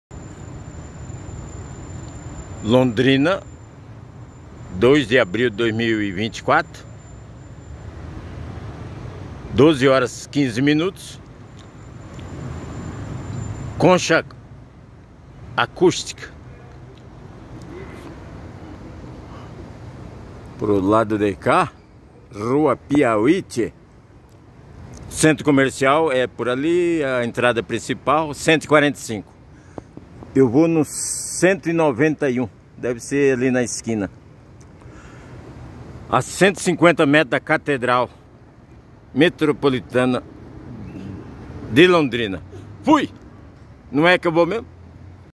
121440-Londrina PR-Concha Acústica-Centro Cial- Catedral Sg Coração de Jesus-Calçadão-02/04/24.p.1/6
Bora conhecr mais um pouco do centro de Londrina? Bora...
Obs.1: Desta feita o Ponga adentrou no recinto da Catedral, a qual é dirigida pelo Arcebispo Metropolitano dom Geremias Steinmetz.
Obs.2: já no calçadão entrevistamos um poeta londrinense de 80 anos, o senhor Vardemiro, que já residiu há mais de 60 atrás na zona rural do município de São Pedro do Ivai PR.
Uma colaboração de e, em nome de,
a) Adiones Gomes da Silva, Adiones Gomes Da Silva Ponga, e, beteradiotv.sitedaradio.com.br